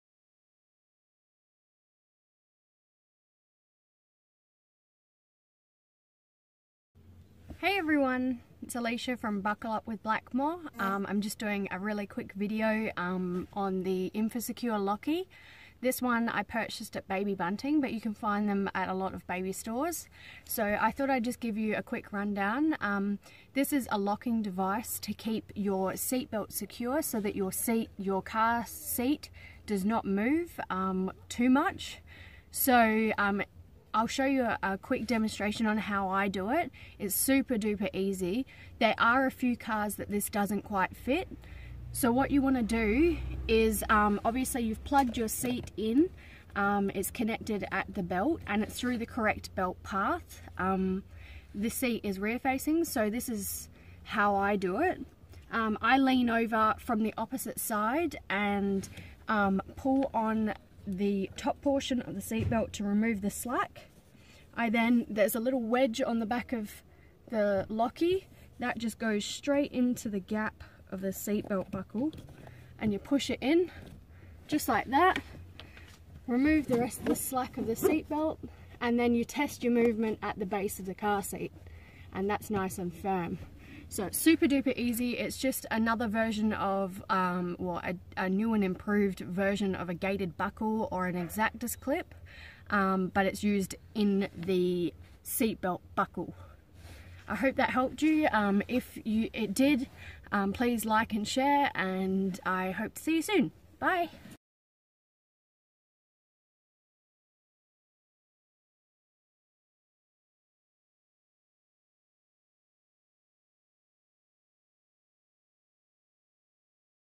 《 INFASECURE LOCKIE 》 (Installation Demonstration)
Facebook: Buckle Up With Blackmore

Type 1 Child Car Seat Installation & Education

September 2017